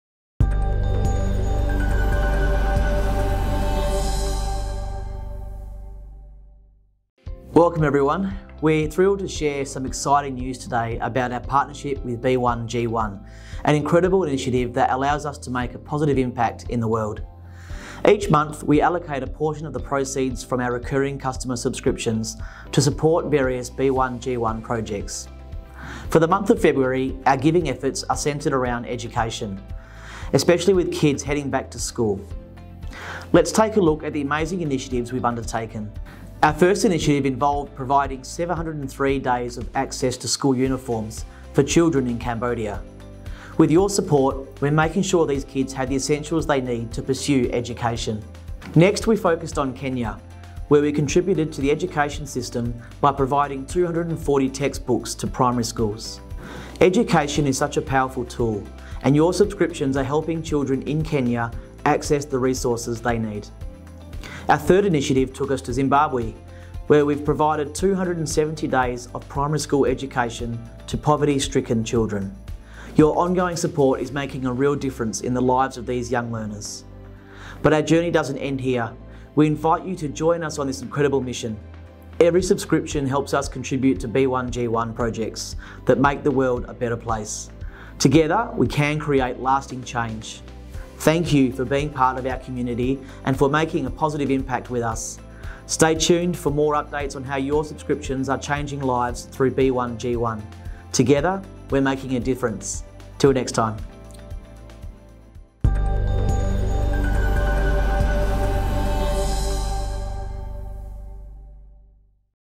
Fleetware Gives Back in February
Every month at Fleetware, we donate a percentage of our subscription income to a variety of different causes through an organisation called B1G1. Brendan Fowler talks through the charities we donated to in February.

For more information about our February giving, read our blog post

To keep up to date with Fleetware's giving efforts throughout the year, head to our About